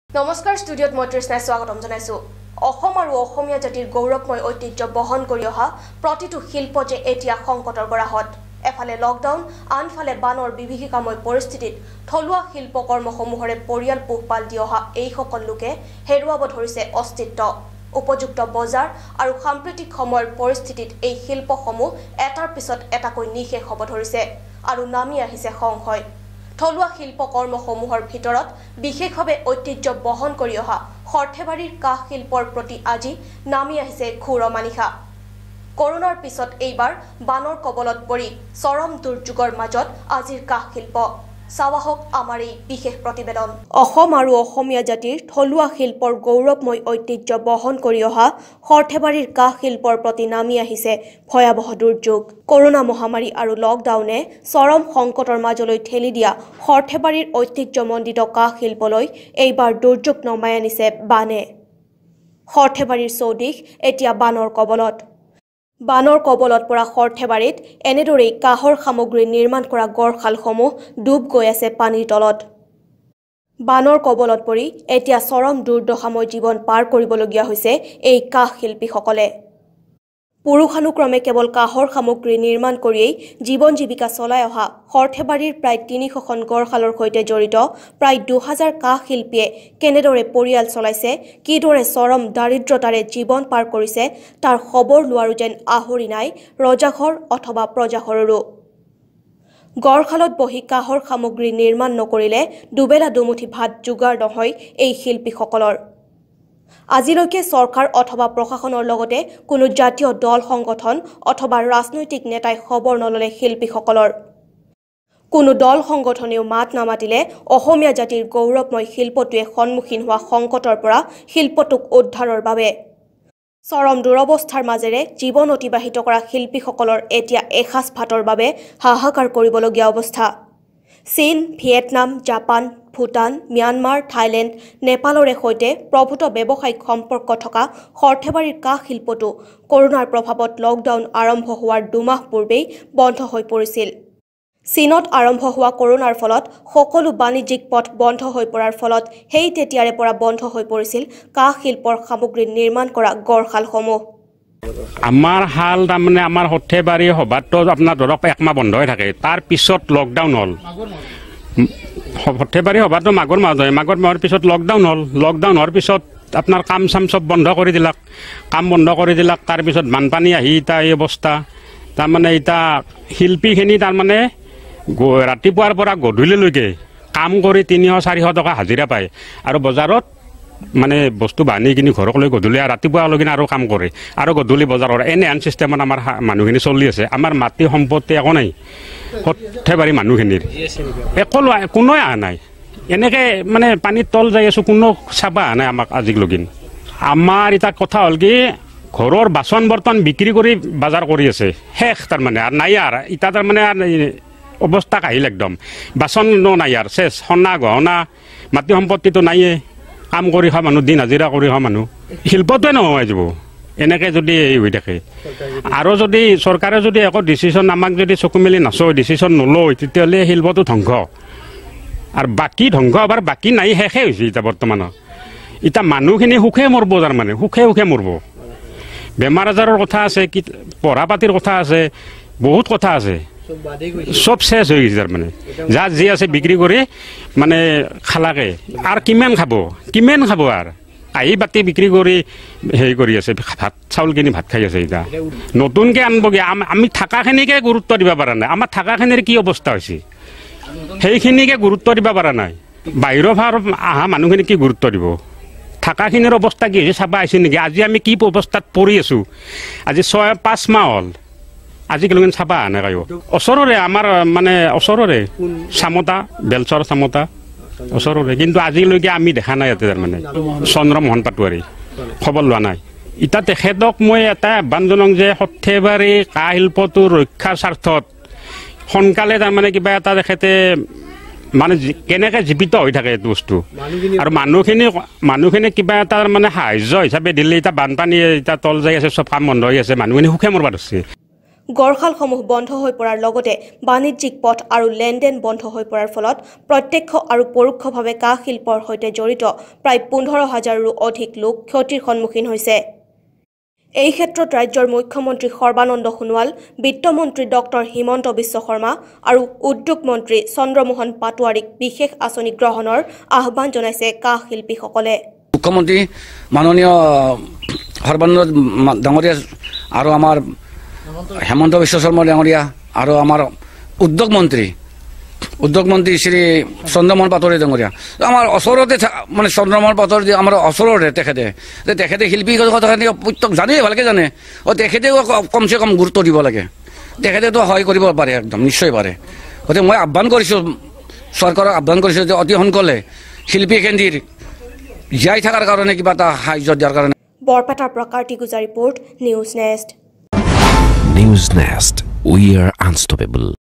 NEWSNEST সৰ্থেবাৰীৰ কাঁহ শিল্পলৈ অশনি সংকেত।
NEWSNEST সৰ্থেবাৰীৰ কাঁহ শিল্পলৈ অশনি সংকেত। দুৰ্যোগৰ মাজত অসম আৰু অসমীয়াৰ ঐতিহ্য বহন কৰা কাঁহ শিল্প।